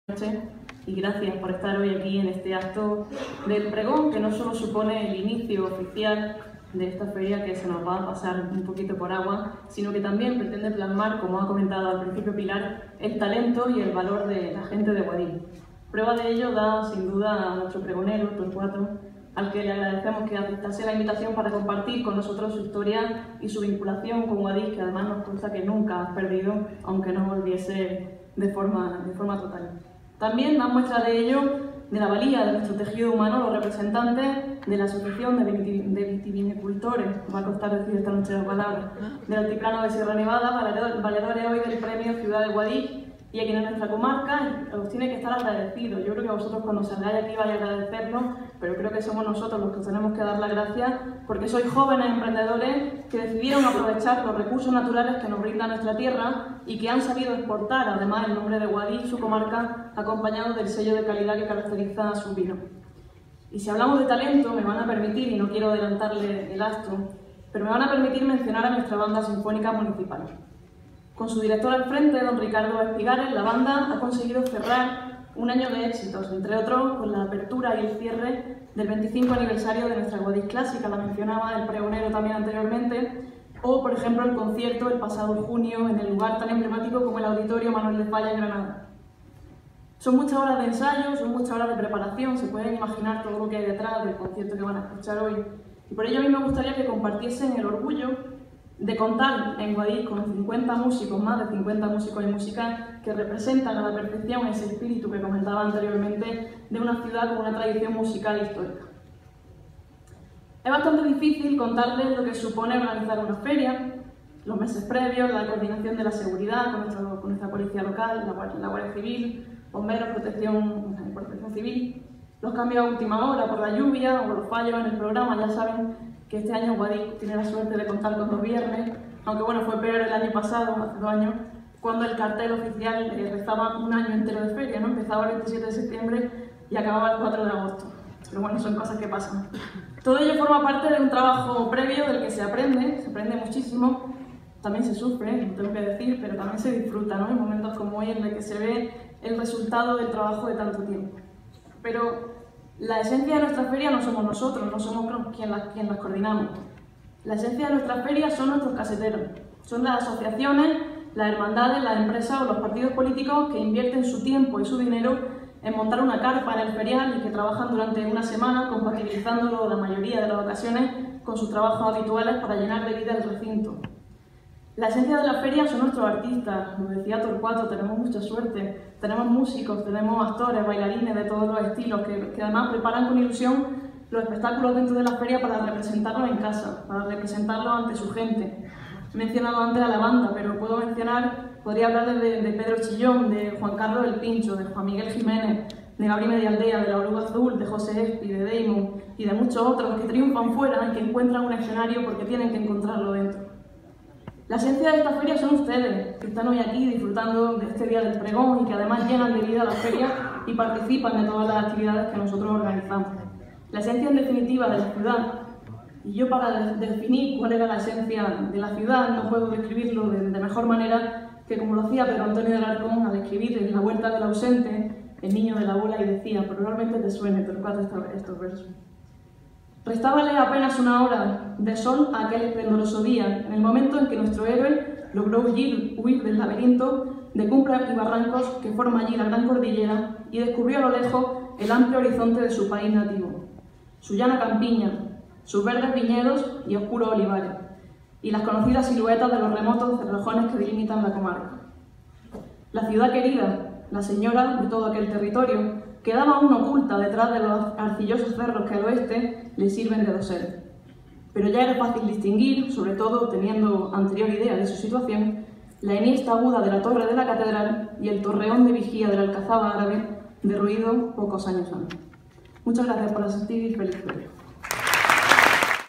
Palabras de la Concejala de Fiestas - Feria de Guadix 2017
Beatriz Postigo, concejala de Fiestas del Ayuntamiento de Guadix, nos habla sobre la Feria y fiestas Guadix 2017 y su preparación.